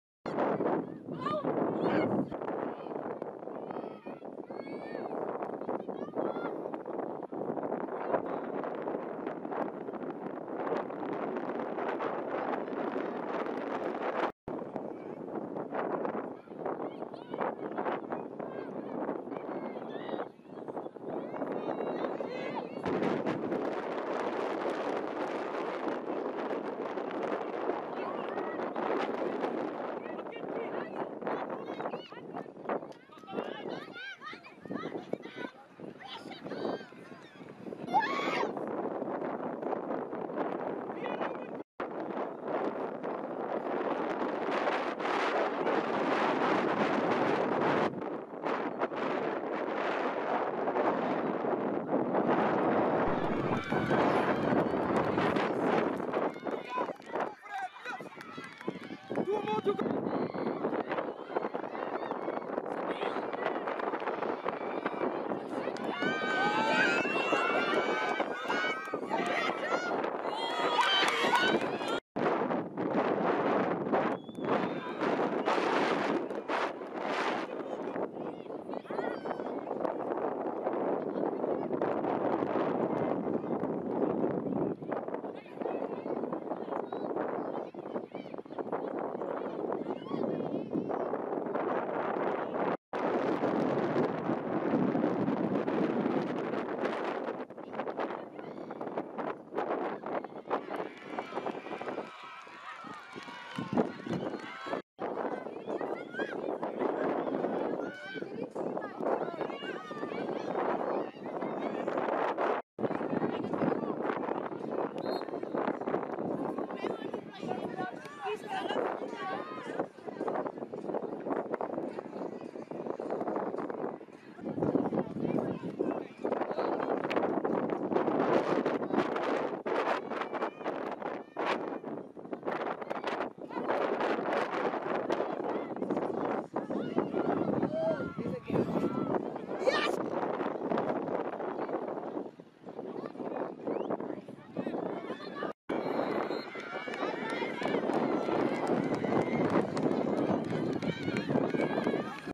Intense Action: Striker vs Defender in Female Cup Soccer Match Salt River Blackpool vs Rygate full
Watch now as two top tier female soccer teams go head to head in a thrilling match filled with intense action. Witness the battle between striker and defender unfold as they showcase their skills in this exciting tournament game. Don't miss out on this nail-biting showdown in women's football!